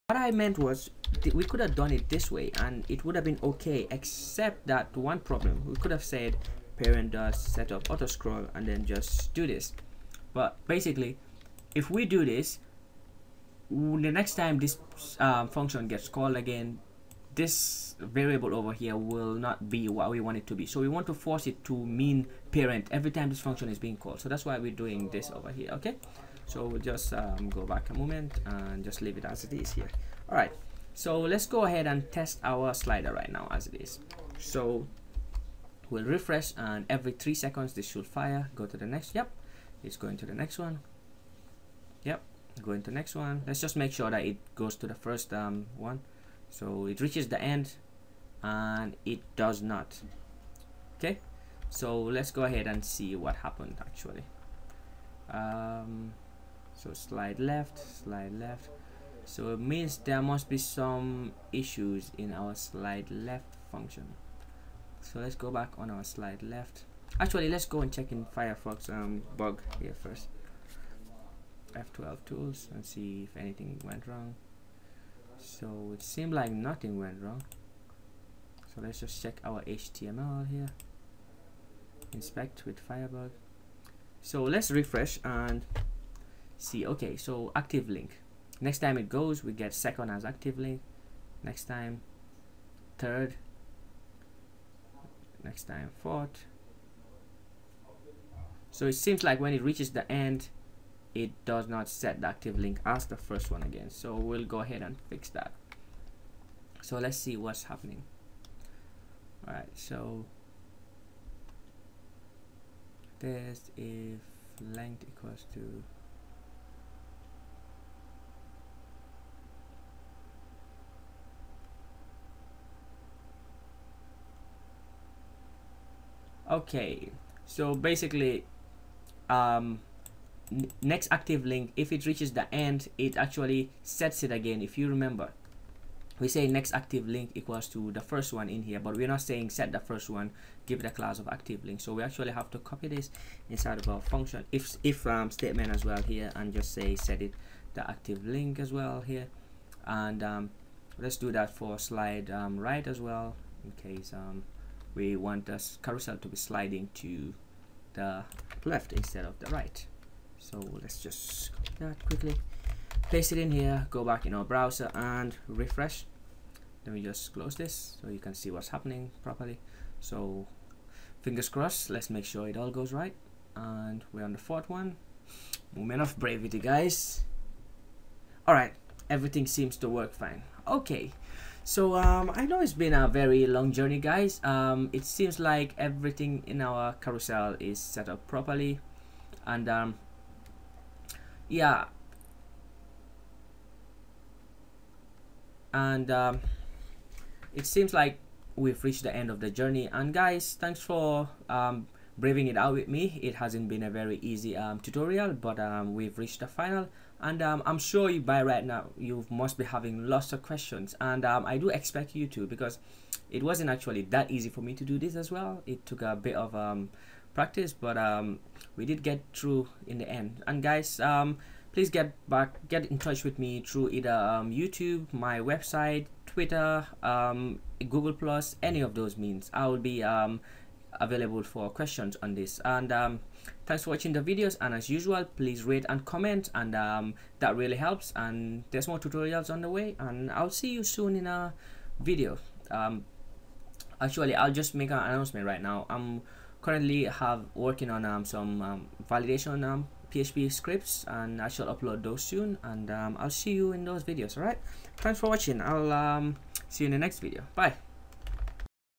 Advanced Jquery Image Carousel Slider - Part 22 - Scroll Buttons G
An advanced jquery slider or carousel plugin tutorial made from scratch using jquery, html, css only.
Heavily customizable, functionality and style wise. Hope you enjoy it and